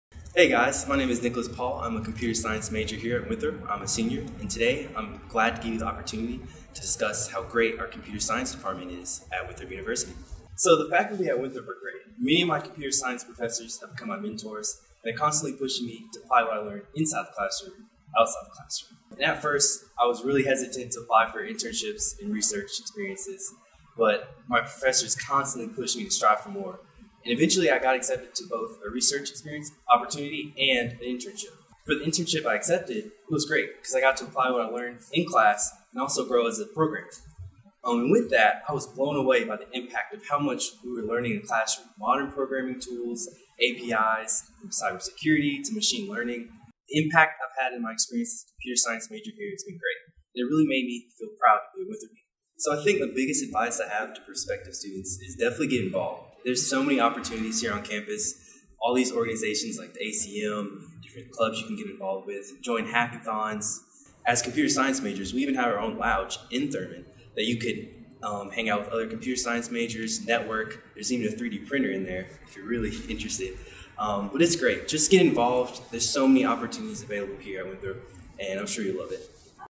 Winthrop CBA Student Stories: Computer Science Nicholas Paul CAP
Winthrop student Nicholas Paul shares personal accounts of how Winthrop College of Business has enriched his overall educational experience.